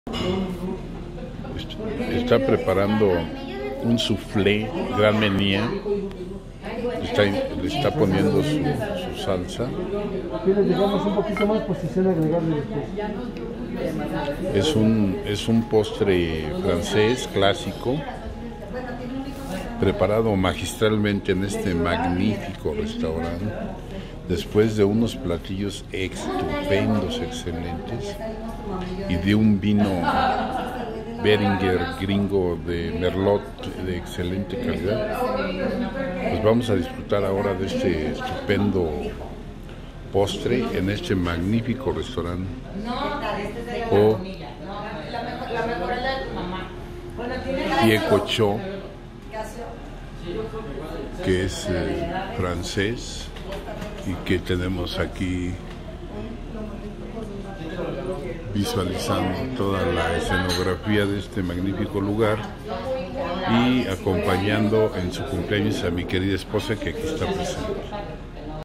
Soufleé Grand Mernier
Aud Pie de Cochon, Presidente Intercontinental, Cd. de México. 06 de Diciembre del 2020